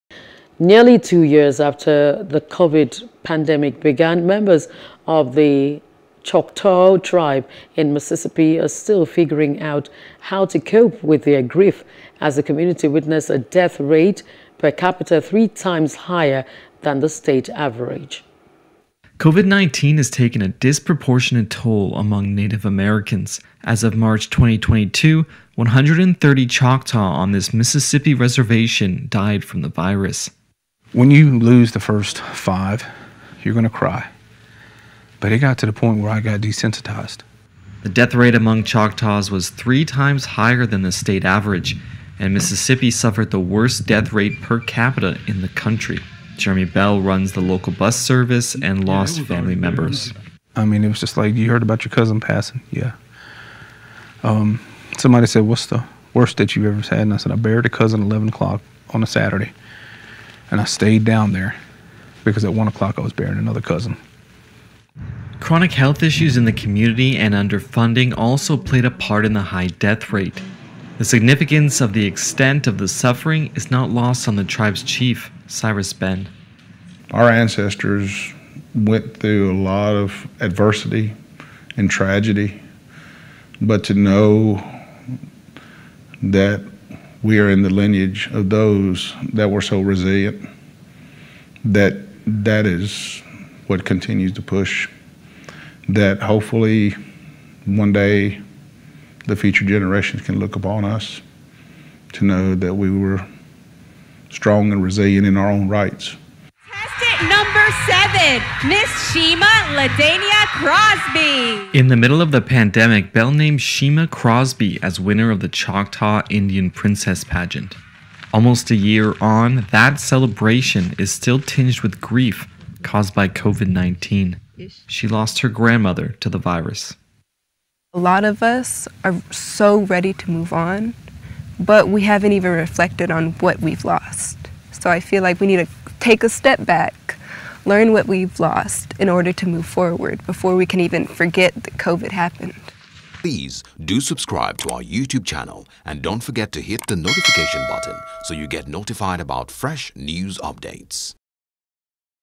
Choctaw Tribe: Native Americans Fight To Keep Traditions Alive After COVID | FOREIGN
Nearly two years after the COVID pandemic began, members of the Choctaw tribe in Mississippi are still figuring out how to cope with their grief as the community witnessed a death rate per capita three times higher than the state average.